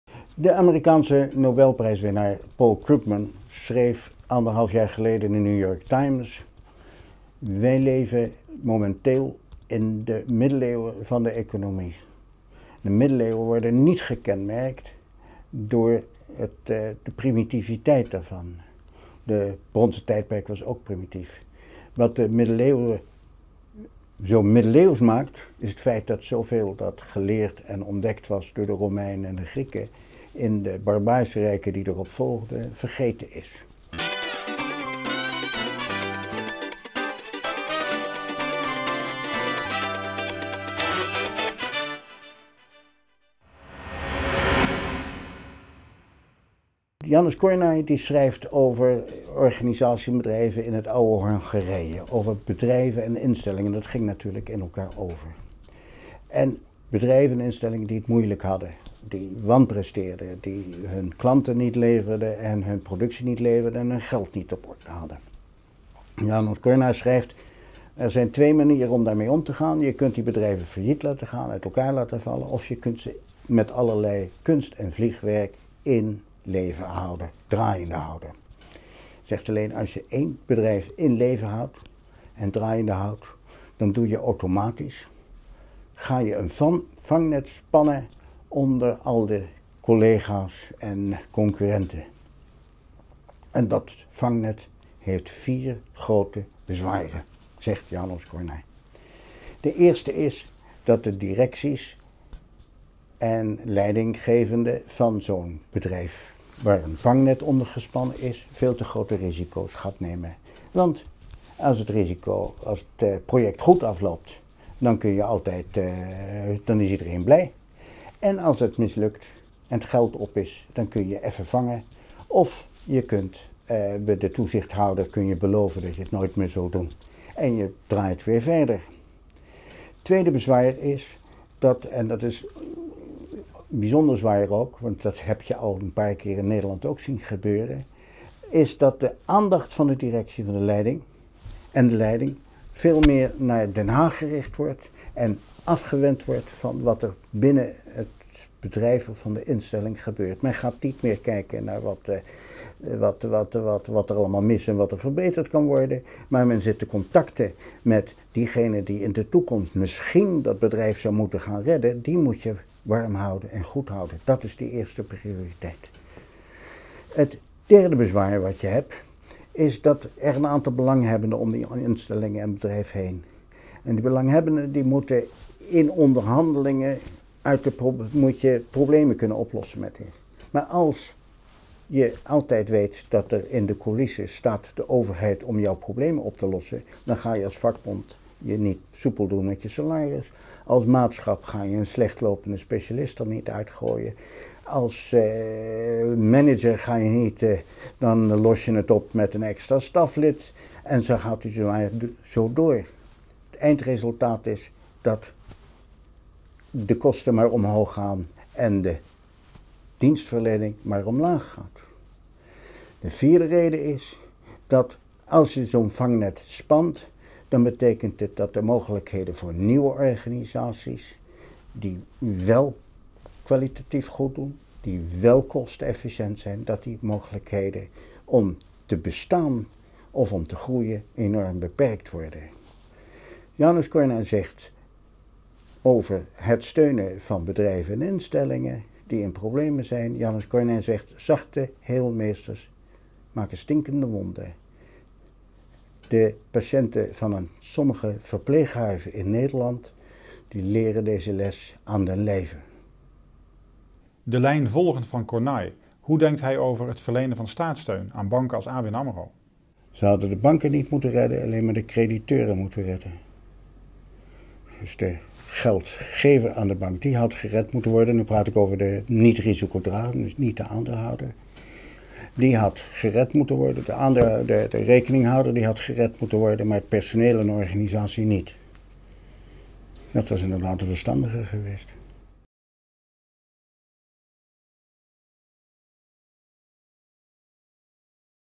Me Judice - Huigh van der Mandele over János Kornai
Arjen van Witteloostuijn en Huigh van der Mandele hebben een boek geschreven: "Economen uit het verleden over de crisis van het heden", geïnspireerd door Paul Krugman. Van elf economen en hun theorieën wordt een beeld geschetst; wat valt er in deze tijd te leren van het gedachtegoed van economen uit vervlogen tijden.